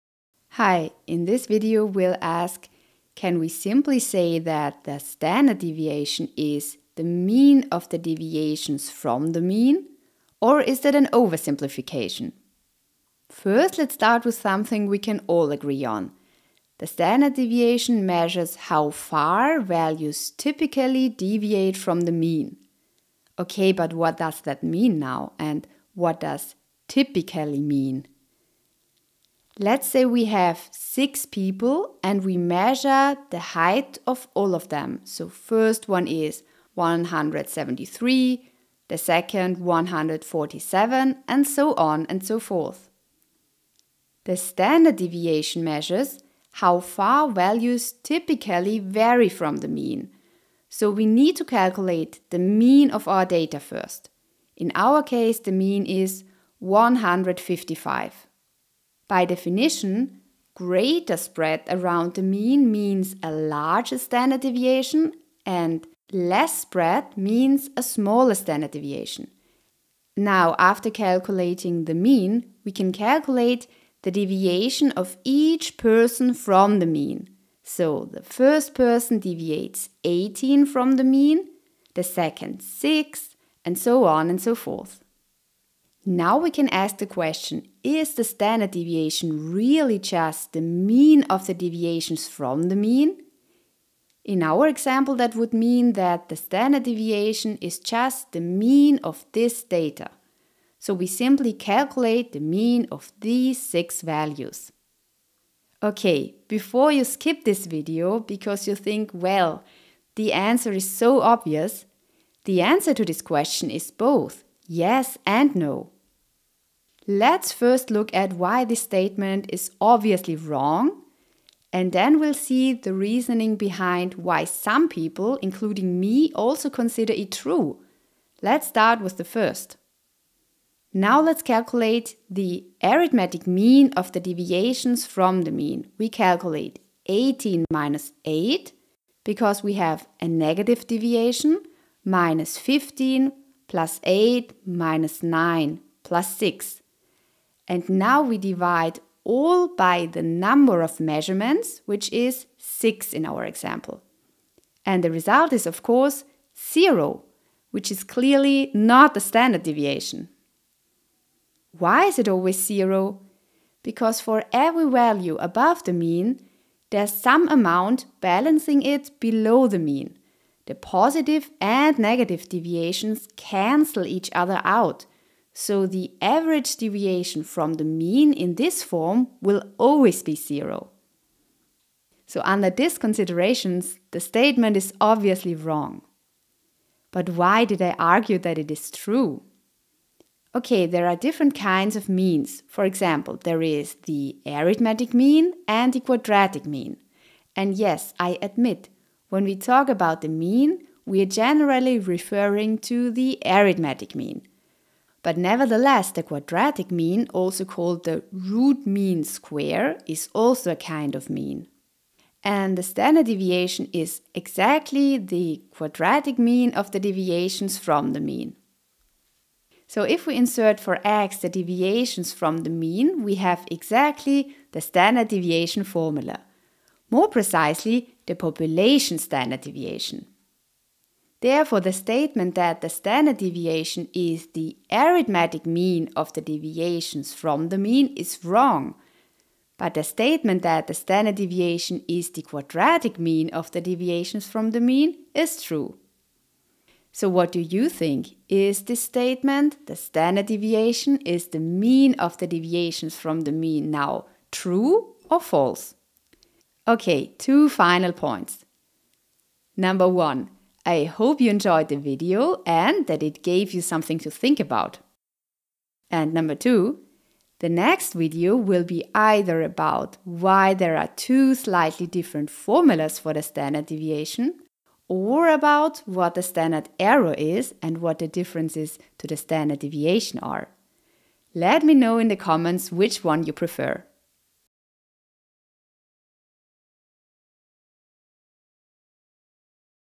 Can we simply say that the standard deviation is the mean of the deviations from the mean?
Hi! In this video, we’ll ask: can we simply say that the standard deviation is the mean of the deviations from the mean, or is that an oversimplification?

First, let’s start with something we can all agree on: The standard deviation measures how far values typically deviate from the mean. Ok, but what does that mean? What does typically mean?